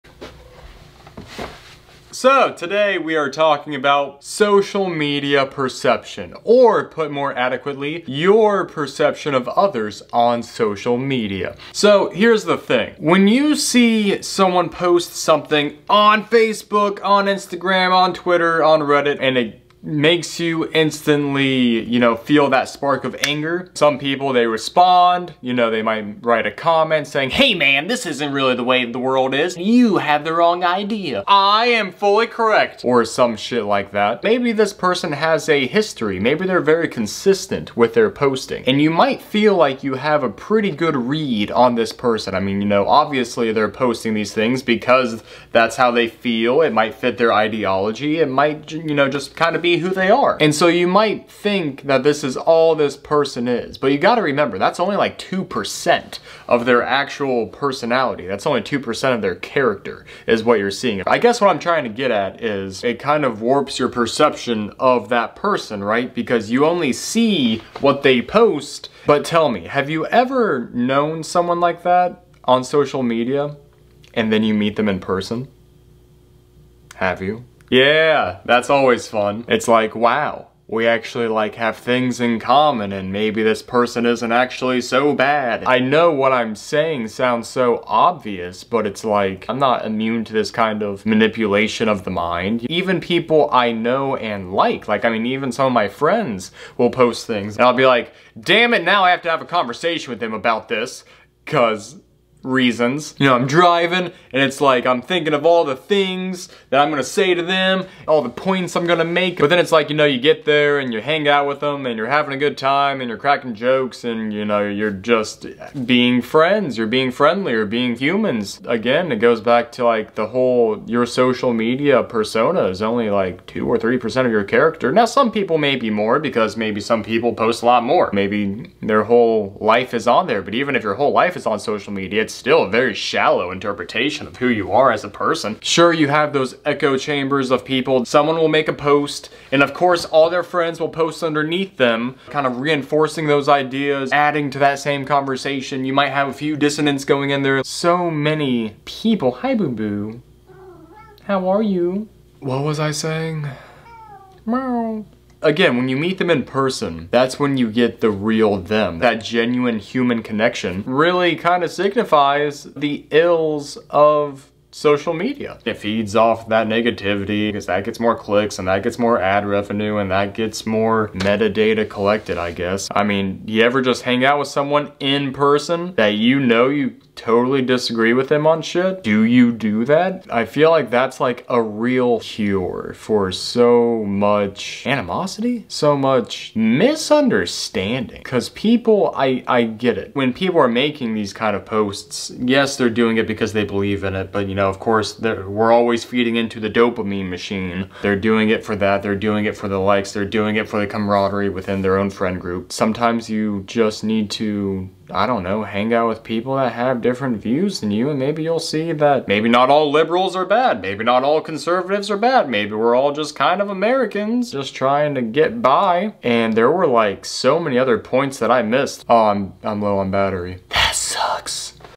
Your Perception of Others on Social Media
Hello! Hope everyone is doing well. So this is a not-as-polished sequel to the "Historical Usage of Social Media" video, talking a little bit about how social media can potentially warp your perception of other people.